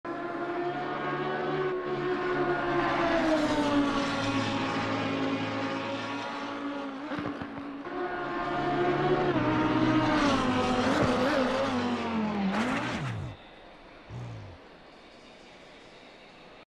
Lamborghini Huracan GT3 Evo Pushing too Hard, Gone Wrong🤦‍♂️ .
Watkins Glen practise , pushing to the limit and resulting in a car crash, literally. Lamborghini Huracan GT3 Evo Crash. Woops.